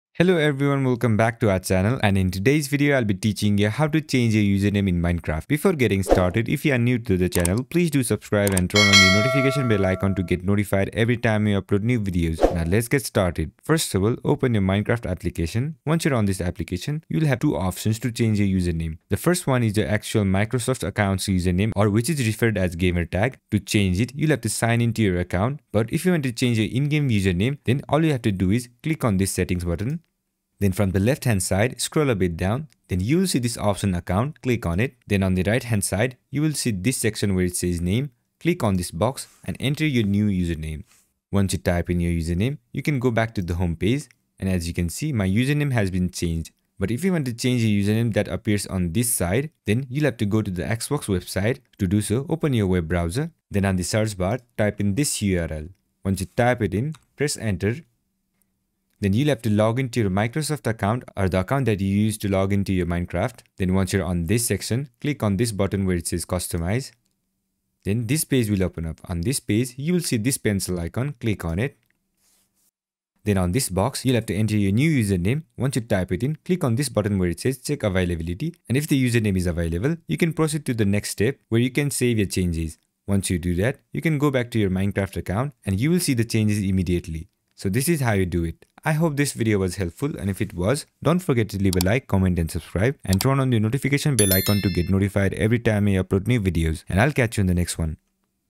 How to Change Username on Minecraft (2024)
Learn how to change username in minecraft
Sick of your old Minecraft handle? Itchin' to shed your noob skin and roam the blocky world with a fresh identity? This ultimate guide to changing your Minecraft username has you covered!